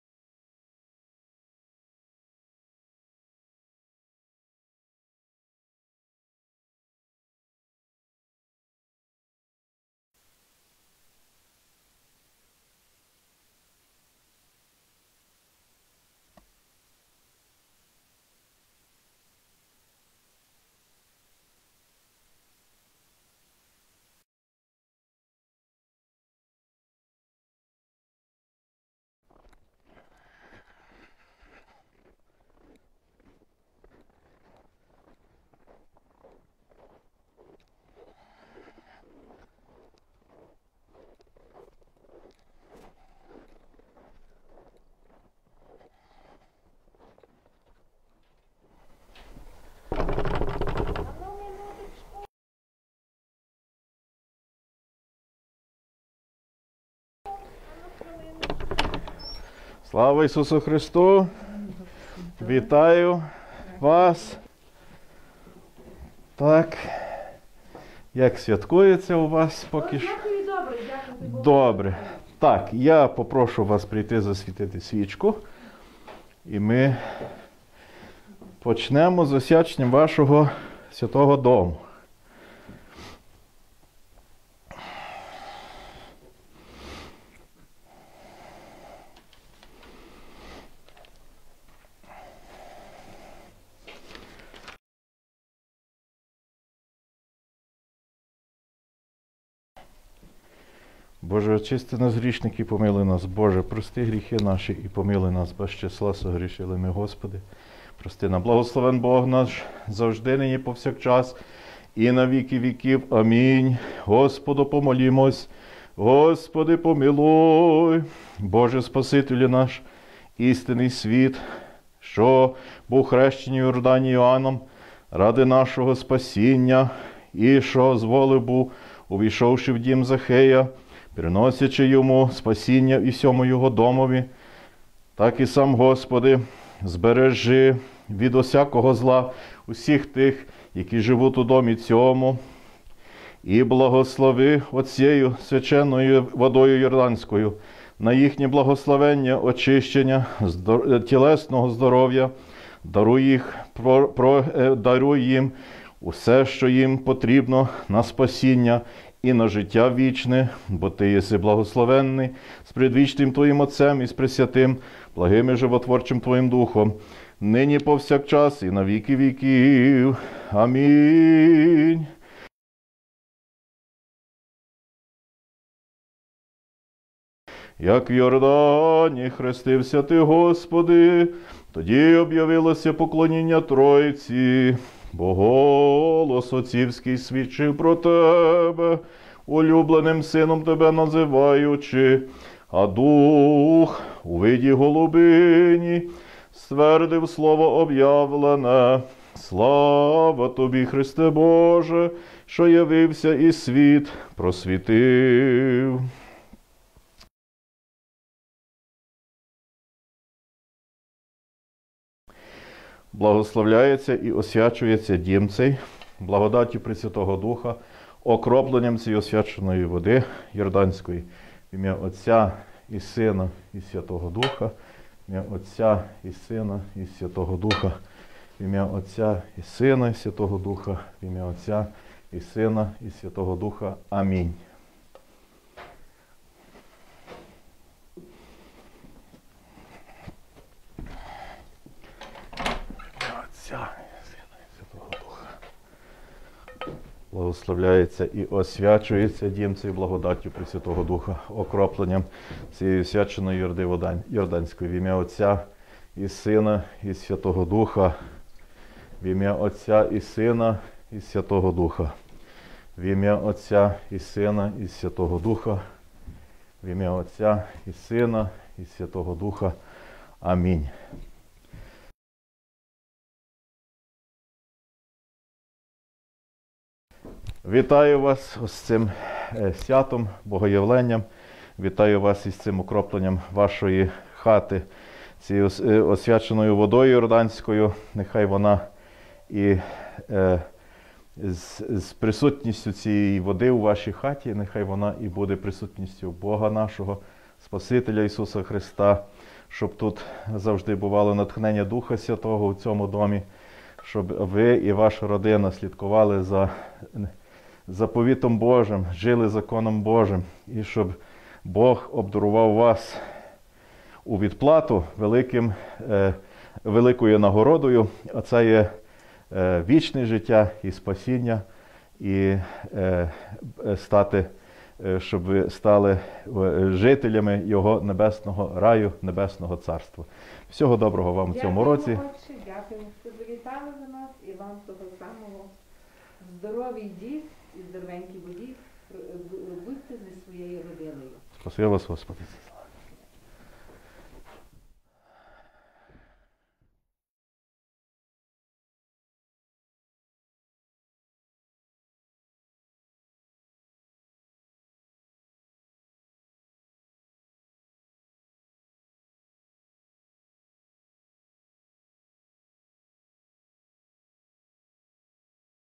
Home Blessing Video (Ukrainian)
Beginning on the afternoon of January the 19th or on the days that followed, parish priests visited the homes of the parishioners to bless them with the Holy water blessed on Iordan. Watch Archpriest Cornell Zubritsky bless one of the homes at the Ukrainian Cultural Heritage Village.